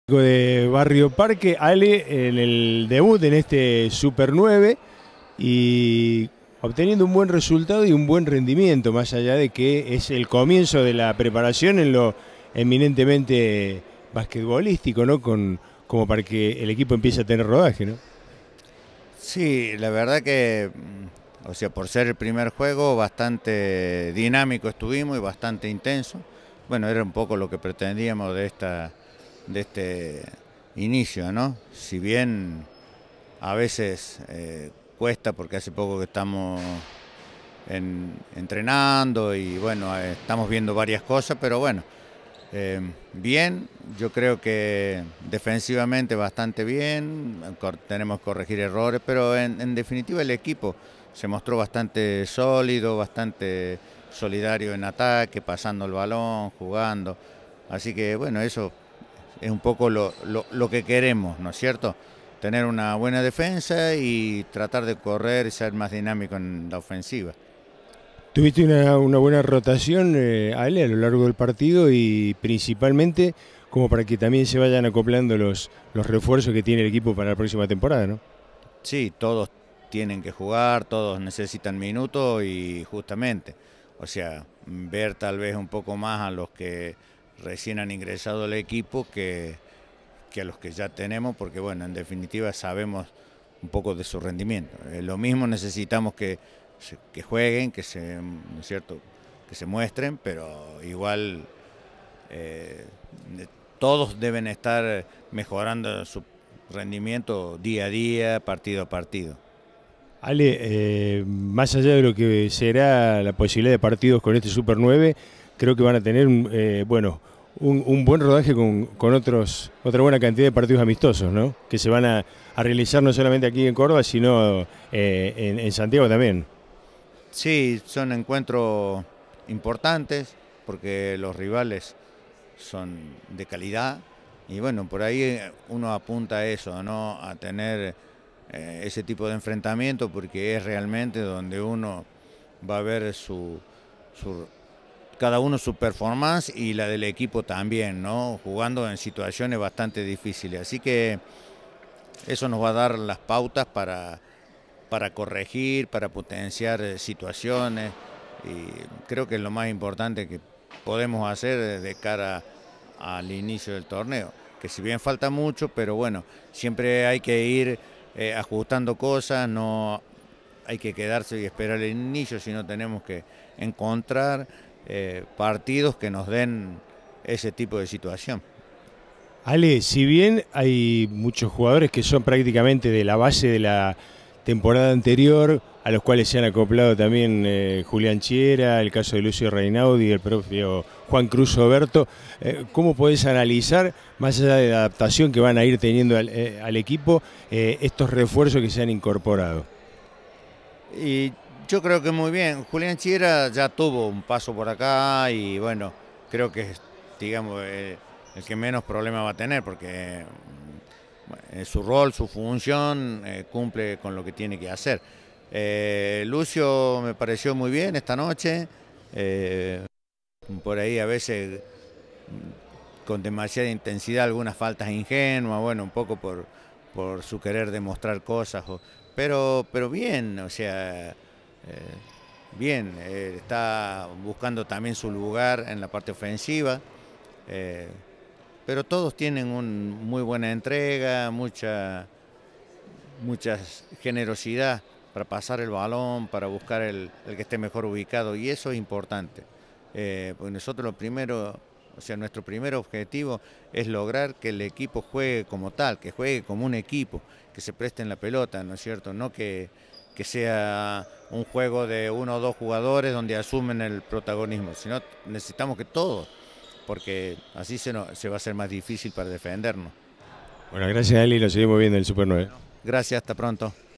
Alejandro Lotterio DT Barrio Parque Pretemporada Liga Argentina
Nota realizada en el marco del Súper 9 que se juega en Córdoba